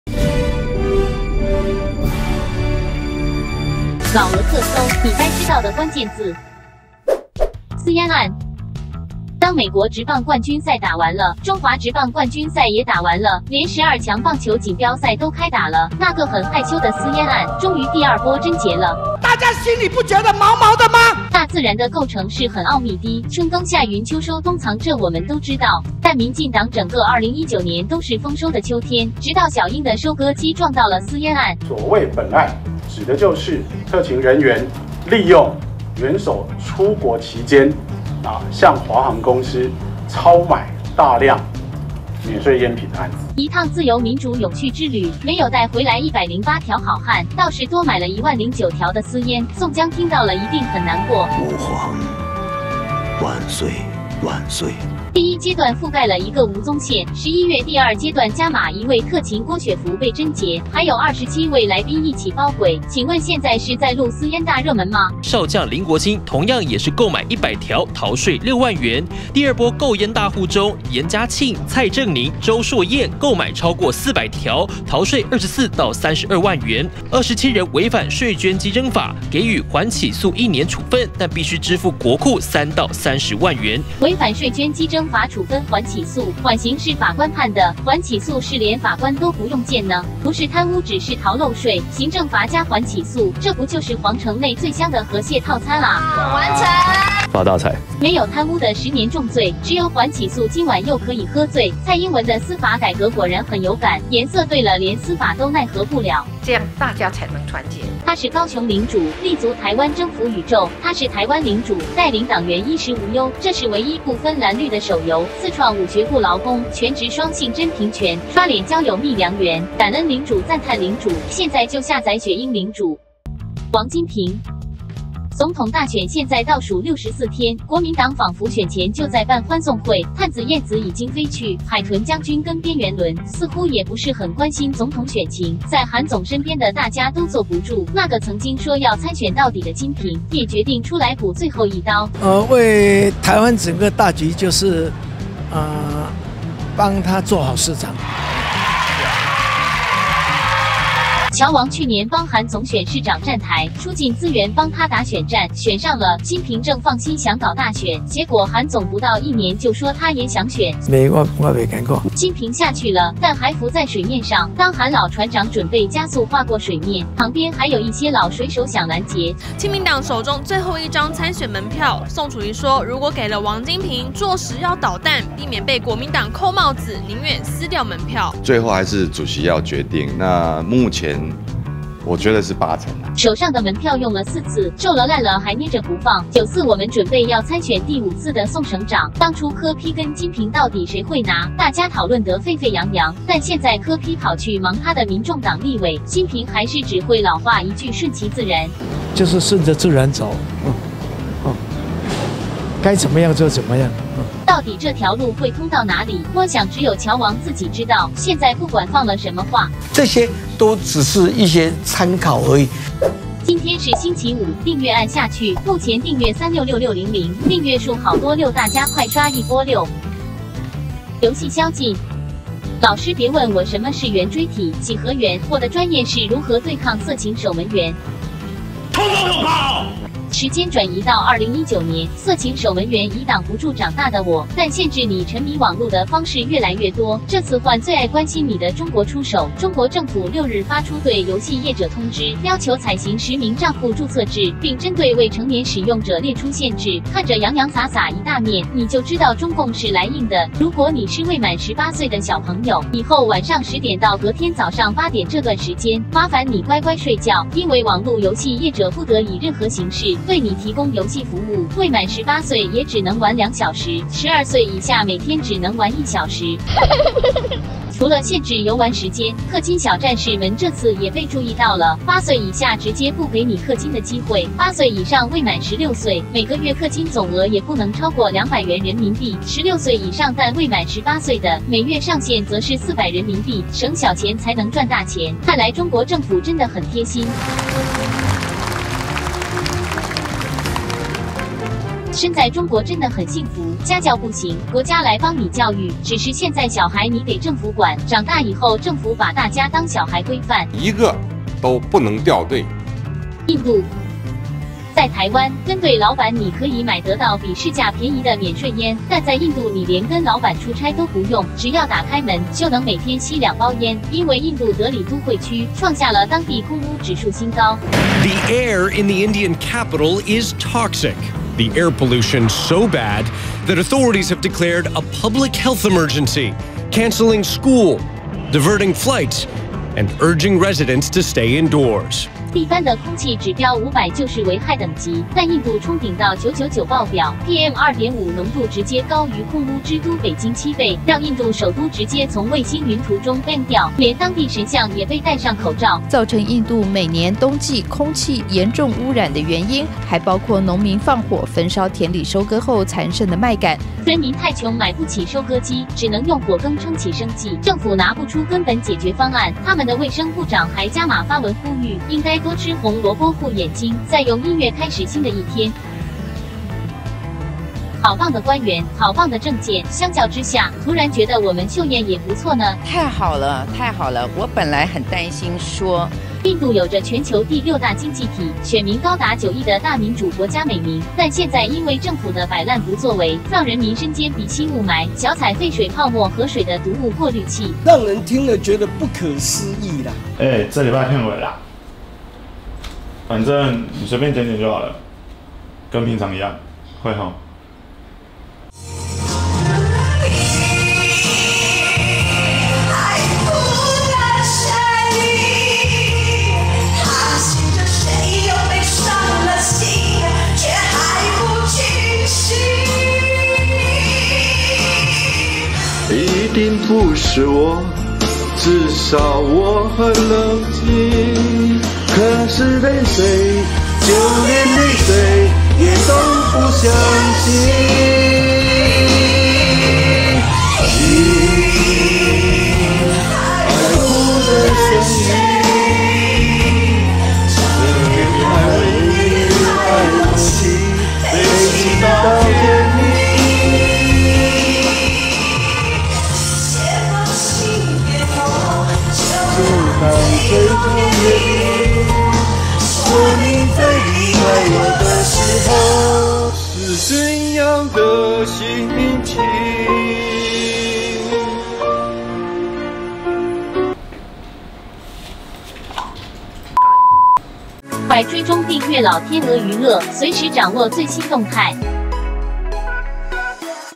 老鵝特搜#224 私菸案／王金平／遊戲宵禁／印度
▶ 你以為大家都忘記私菸案了嗎
▶ 這次又是哪些倒楣鬼被包軌
▶ 民進黨的私菸大熱門

▶ 選前就在辦歡送會的KMT
▶ 載浮載沉的金平還在水面等機會
▶ 宋伯伯手上的門票都快被捏爛了

▶ 以後玩遊戲遇不到對岸小弟弟了嗎
▶ 94讓你沒得課也沒得玩
▶ 不管幾歲政府都把你當小孩管

▶ 在印度一開窗就給你滿滿的免錢菸
▶ 政府無賴民眾受害的最佳典範
▶ 高官484覺得只有平民有肺啊?

「老鵝特搜」每週一到五出刊 趕快訂閱YT頻道